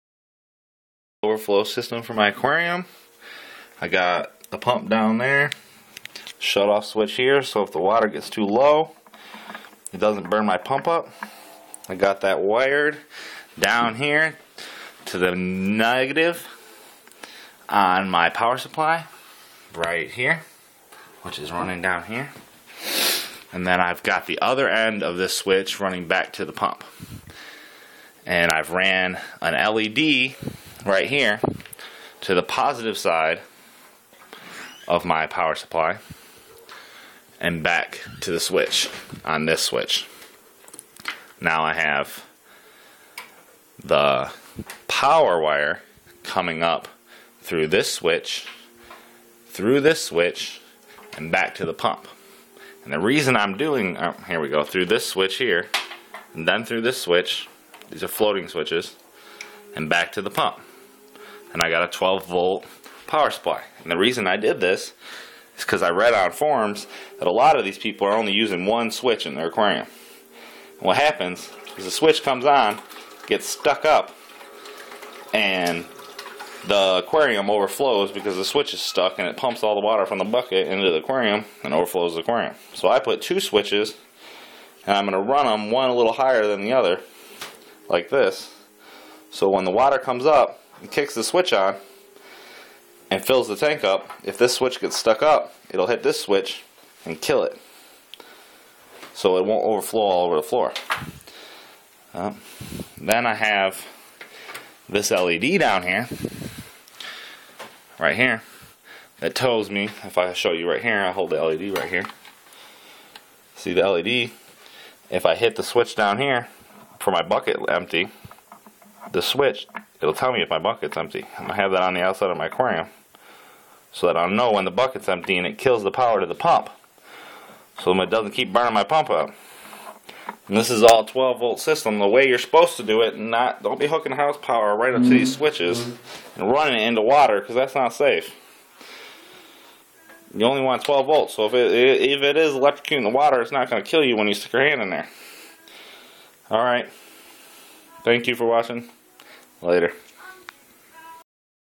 DIY aquarium fish tank auto top off system the right way 12v DC not 110v AC
how to make you own aquarium top off system cheep here is a wiring diagram for my top off system i used tubing 5 gallon bucket float switch x3 12v pump led and 12v power supply I will update this video when i have it hook up to my fish tank ty for watching pls subscribe

all from ebay
FLASHING SOLID BLINK GREEN FAKE CAR ALARM LED 1$

3PCS Liquid Water Level Sensor Right Angle Floating Switch 5$

63GPH 12V Brushless DC Mini Pump For fountain  7$